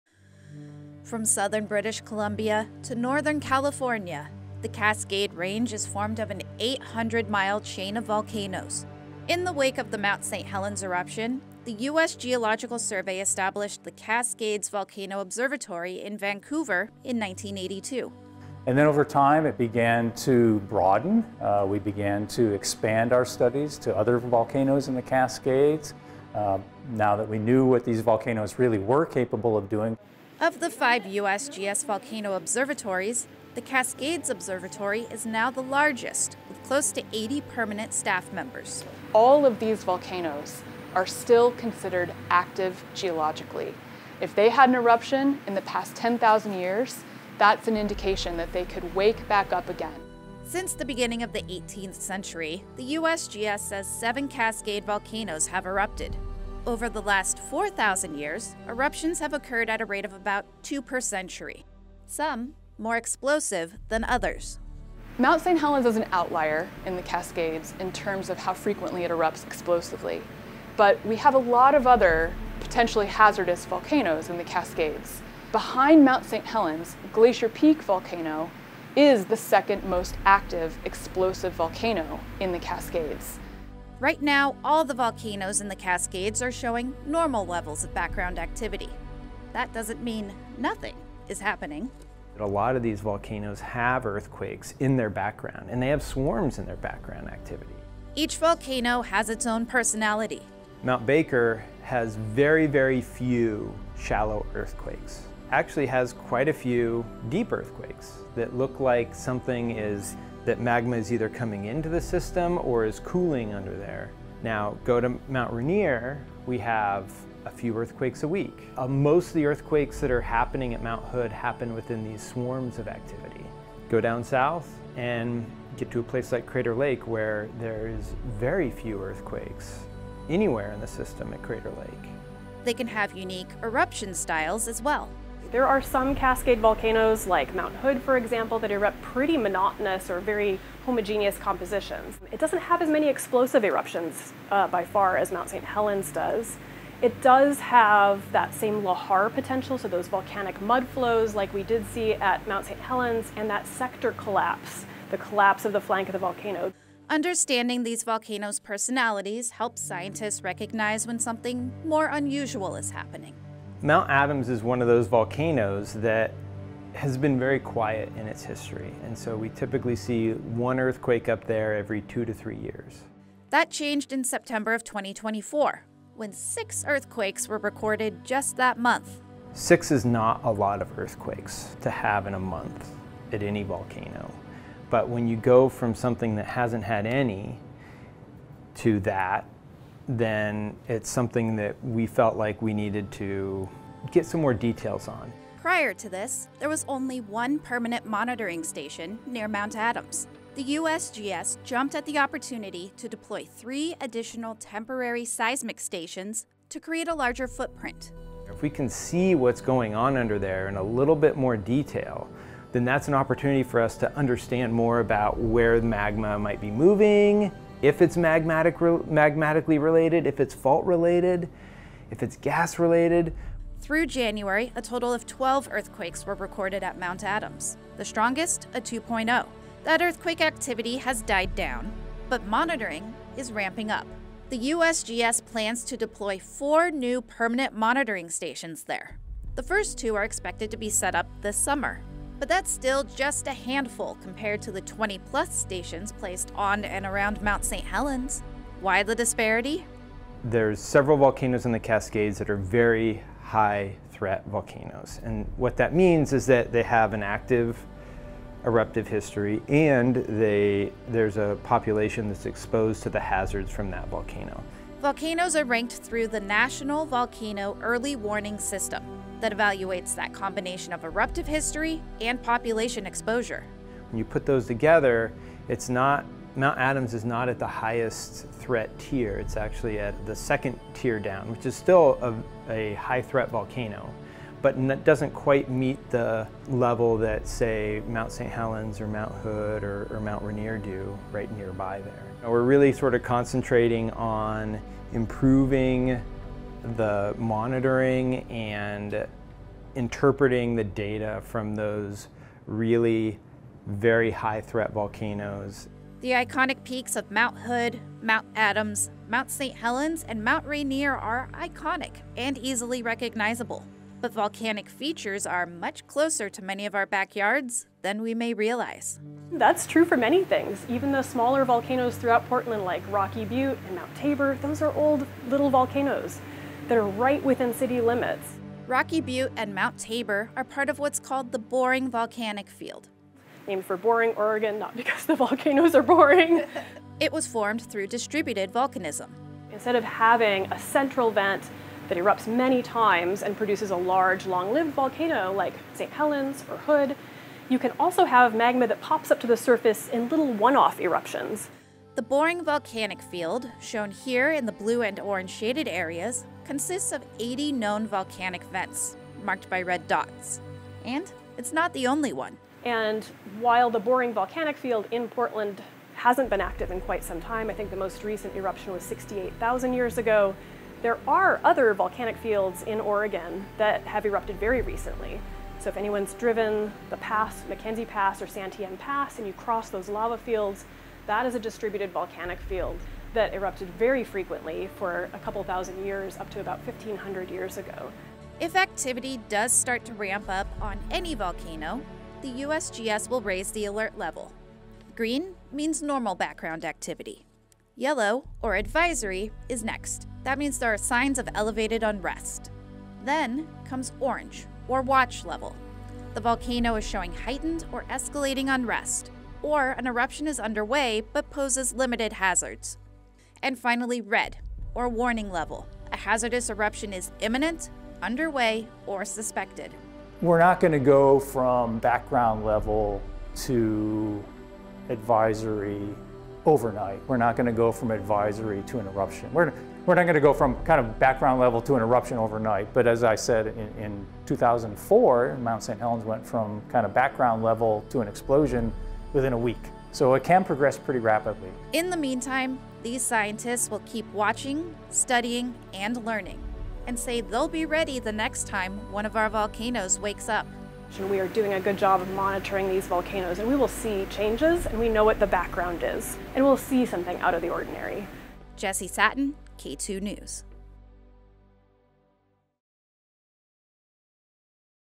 Diverse behaviors of Cascade Range volcanoes unraveled in subduction zone studies
The 1980 eruption cycle made Mount St. Helens one of the most famous and now best-monitored volcanoes in the Cascades. But it is far from the only volcano in the range.

From southern British Columbia to Northern California, the Cascade Range comprises an 800-mile chain of volcanoes. So, how did this volcanic landscape come to be?

“In the Cascades, broadly, we’re located along a subduction zone,” explained United States Geological Survey (USGS) Research Geologist Emily Johnson. “So that's where there's an oceanic plate that's sinking beneath the North American plate. And that creates this chain of volcanoes because as that oceanic plate sinks, it gets heated up and it starts to release fluids and melt from the plate.”